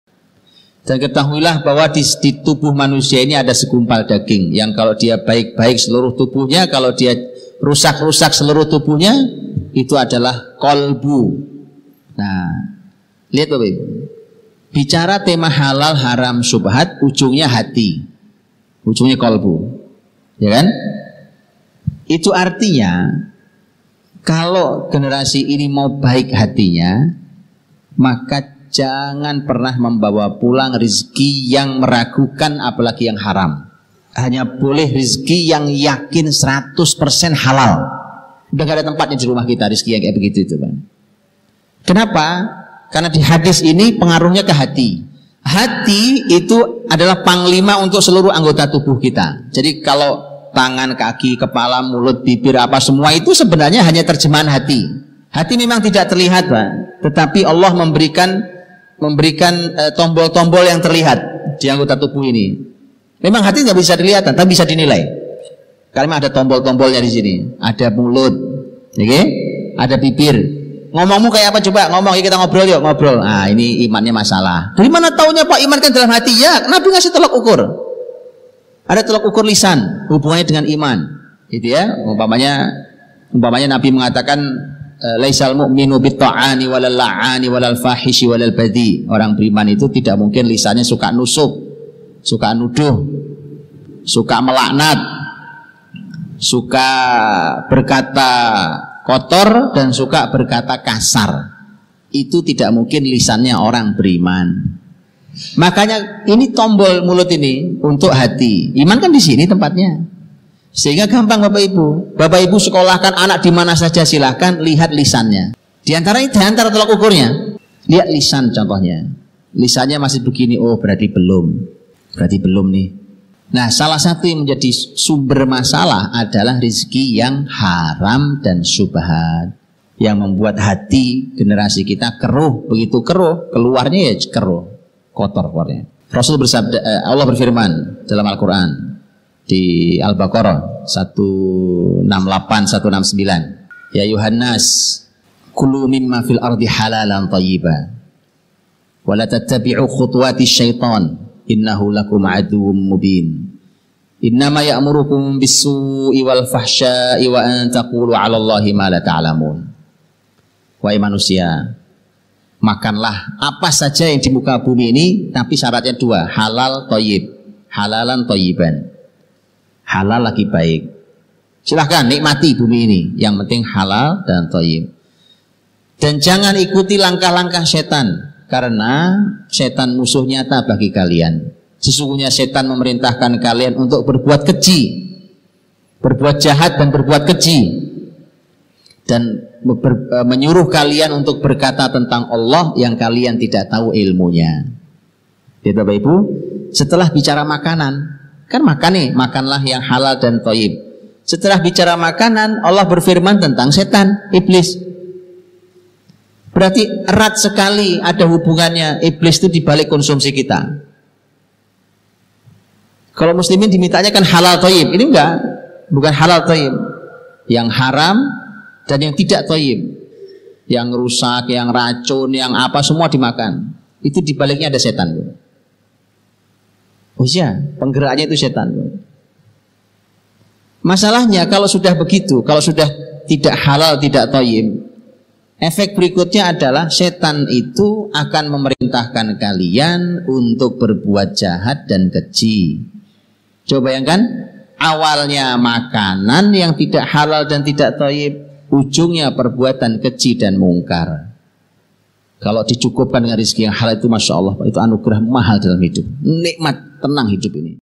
Hidup Tenang Karena Rizki Halal - Ust. Budi Anshari,Lc.
Rizki yang halal pasti membuat hidup tenang.